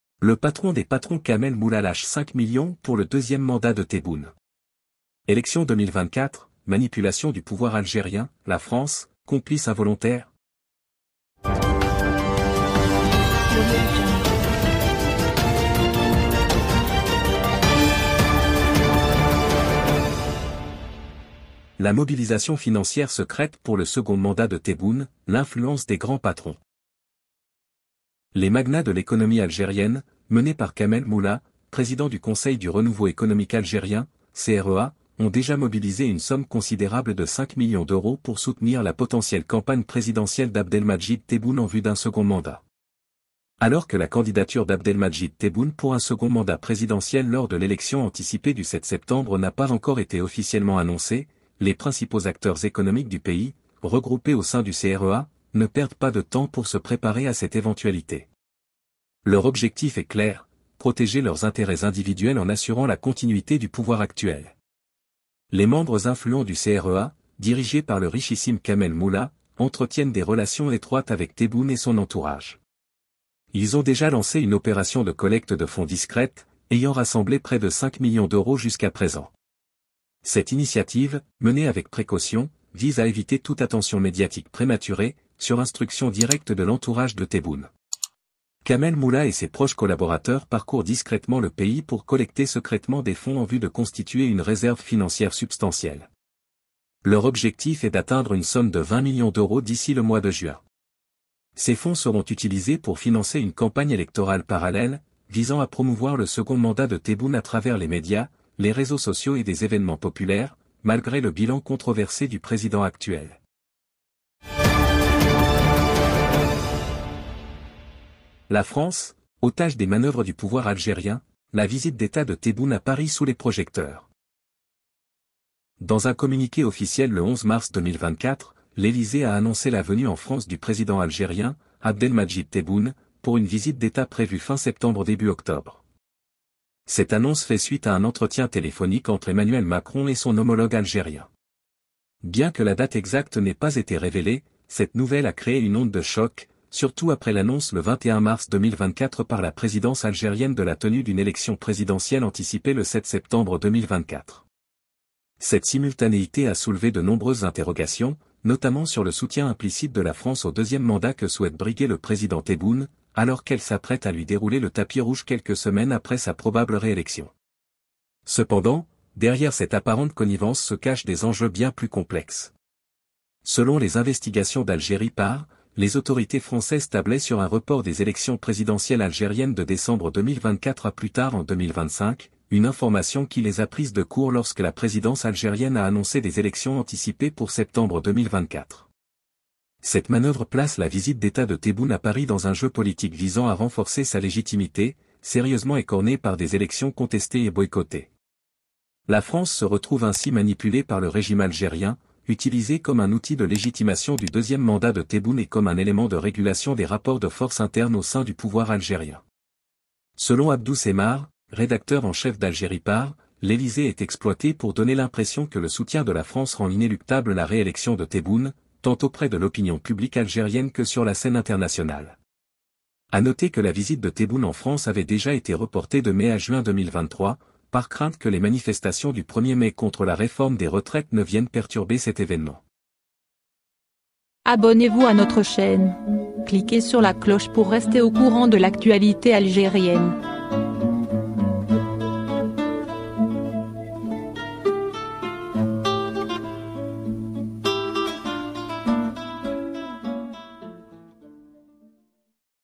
🇩🇿🇫🇷 Tebboune veut piéger la France lors de sa visite à Paris ! 🗳️ 2e Mandat
Découvrez en exclusivité les coulisses du financement secret de la campagne pour le second mandat d'Abdelmadjid Tebboune en Algérie, orchestré par les grands patrons du pays. Quelles implications pour l'avenir politique du pays ? Analyse et révélations dans cette vidéo.
Découvrez les coulisses de la visite d'État de Tebboune à Paris et les manœuvres du pouvoir algérien pour manipuler l'opinion publique, avec toutes les implications politiques sous-jacentes.